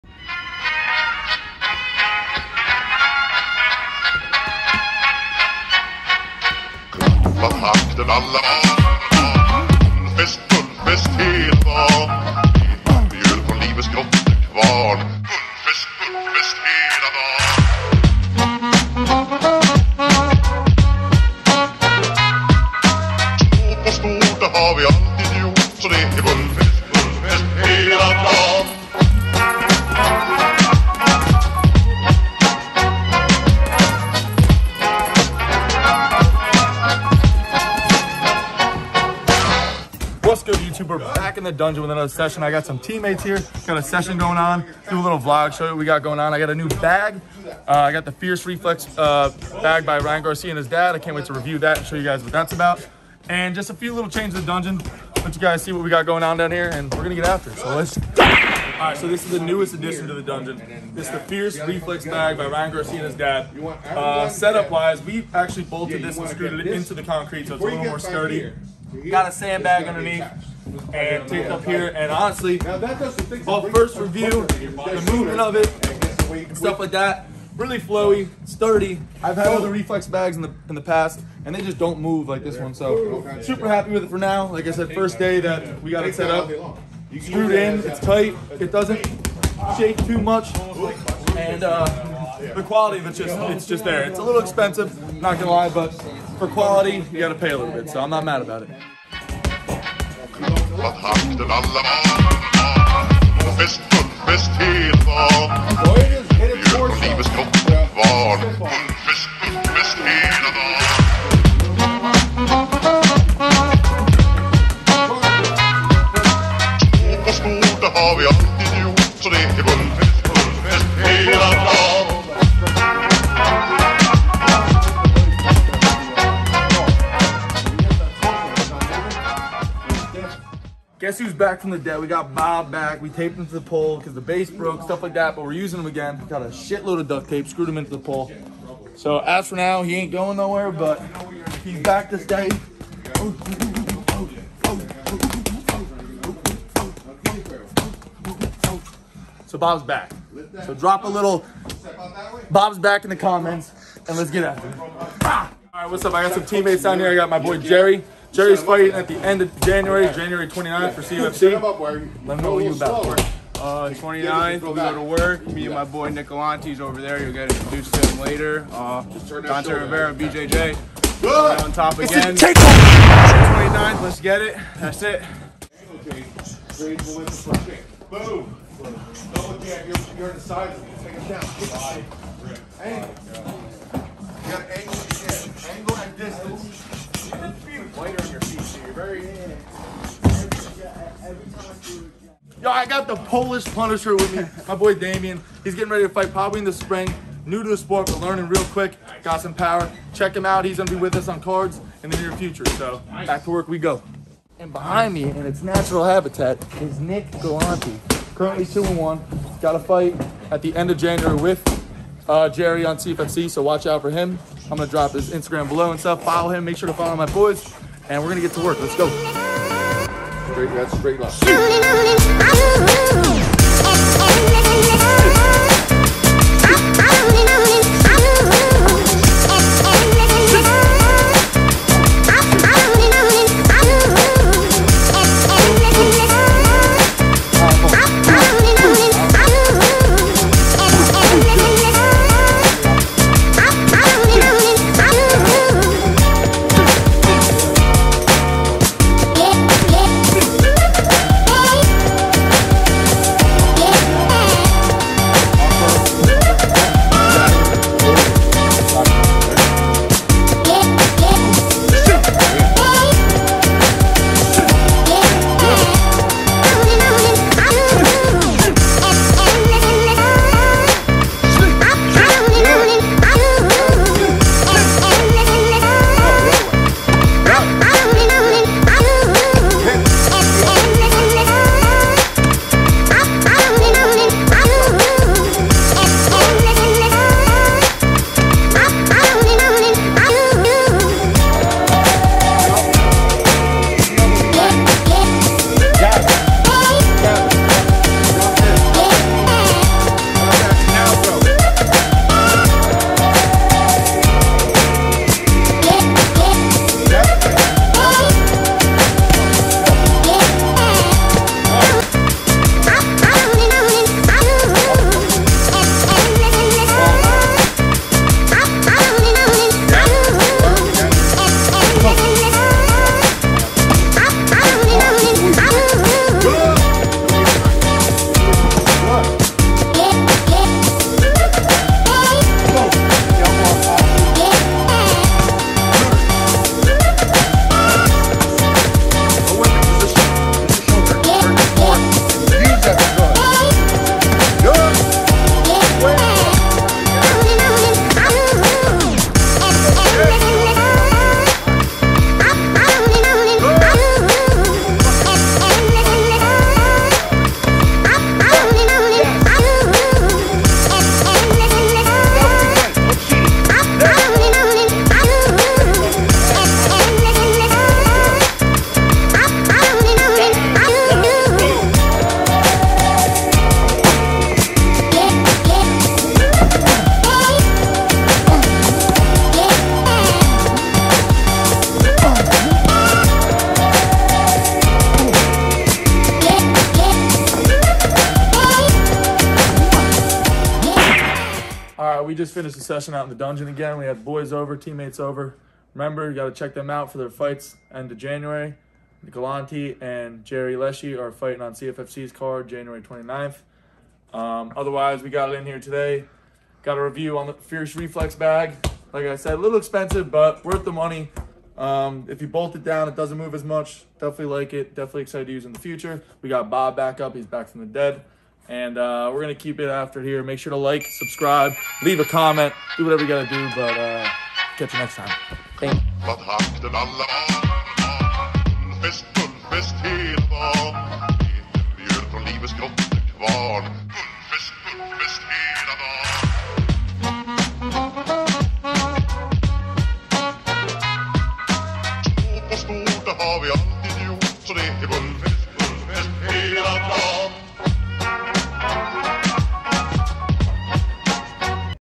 HOME GYM Ep 6: FIERCE REFLEX BAG REVIEW & VLOG
The Dungeon got a few new upgrades since the last video, including Ryan Garcia’s Fierce Reflex bag! Also BOB IS BACK FROM THE DEAD! Check out a solid training session and meet some of my teammates. Make sure to follow them all on Instagram that way you can keep up with their upcoming fights.